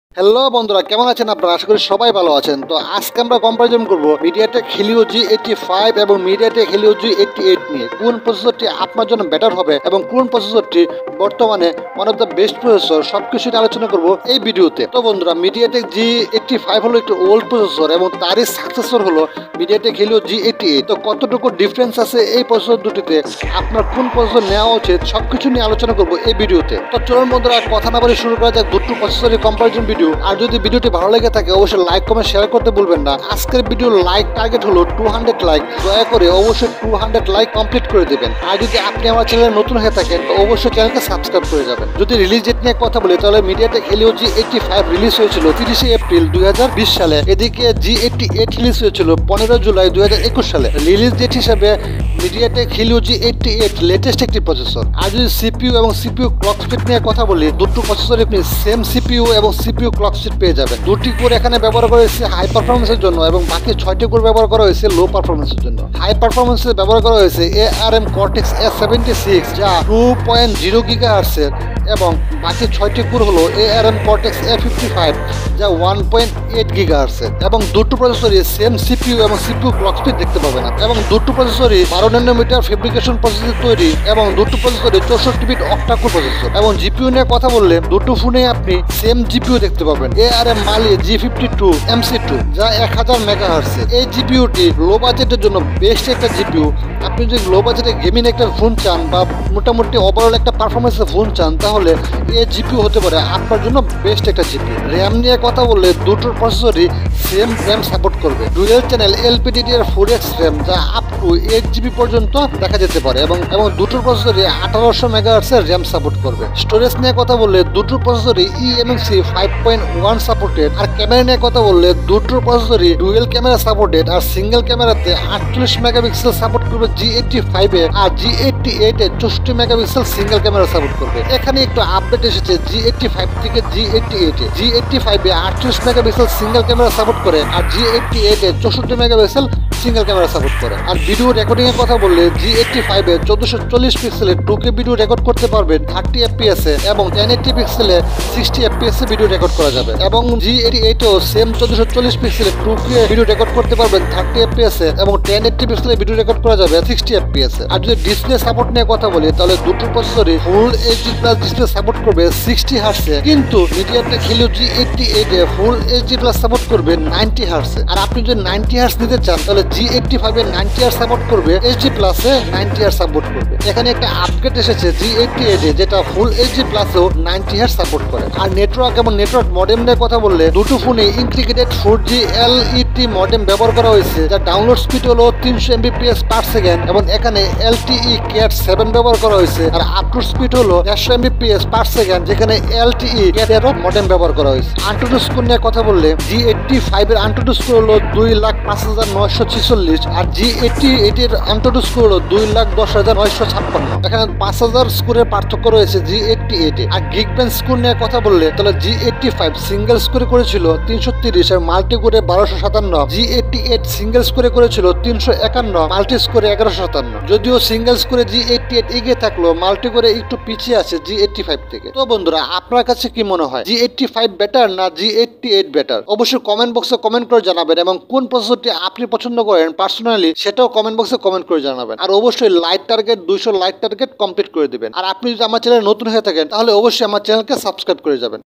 MediaTek Helio G 85 VS MediaTek Helio G 88 Processor | Which is Better? | Realme | Infinix | Xiaomi
MediaTek recently  released thier new processor  mediaTek helio G88. this mediatek Helio G88 is low budget gaming processor. You know that nediatek helio g85 was best low budget gaming processor. recently moto g51 mobe released on india with helio g88. moreover  helio g85 we se many realme, poco, infinix, xiaomi, and other Mobile.